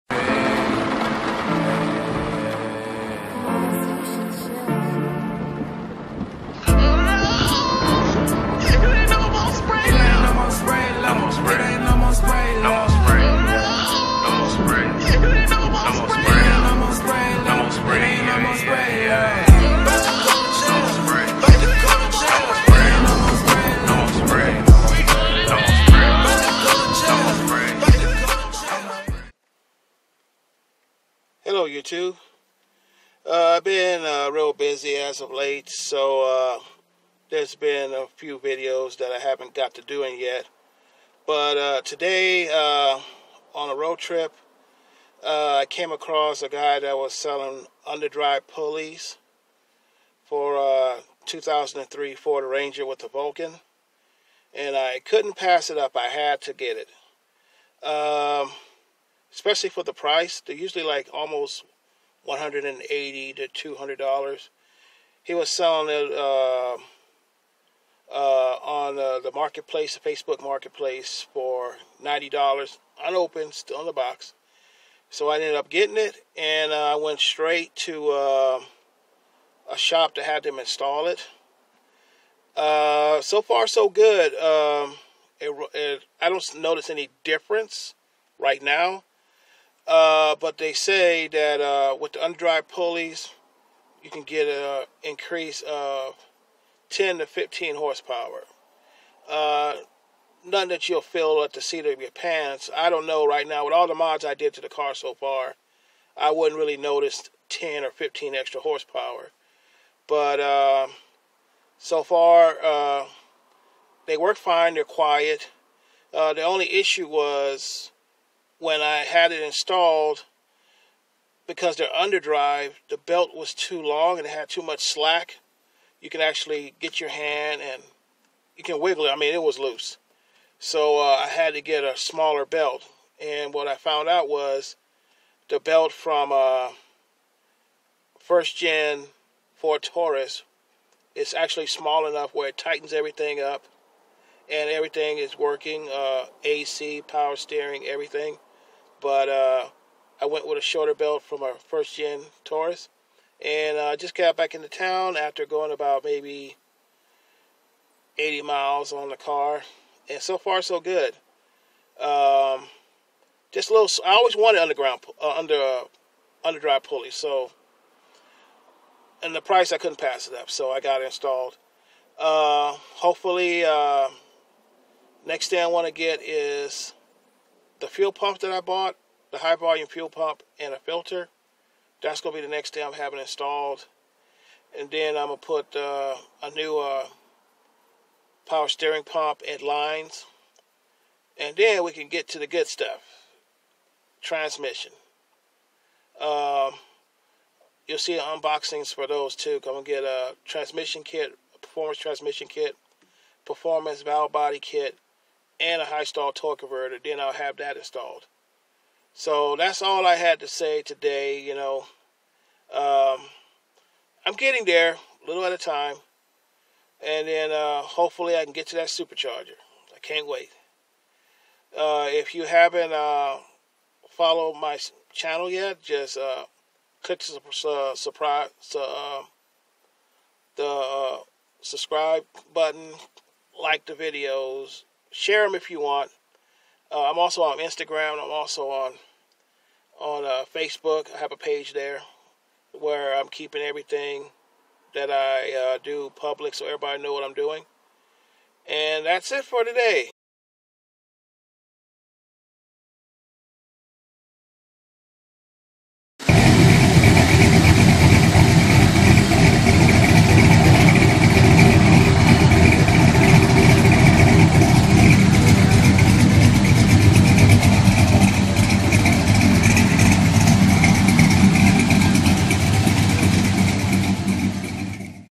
2007 Ford Taurus - Jet Underdrive Pulleys
Came across a deal on a set of Jet Performance underdrive pulleys for a 2003 Vulcan powered Ranger. Had to add spacers to line up the pulleys and a shorter belt from a 1st gen Taurus to make them fit but that work great now. They state it will increase horsepower by 10 to 15 horsepower but I felt no difference myself. Then again it has a lot of mods. Net thing to work on is the fuel pump and filter. Then the head gaskets and injectors.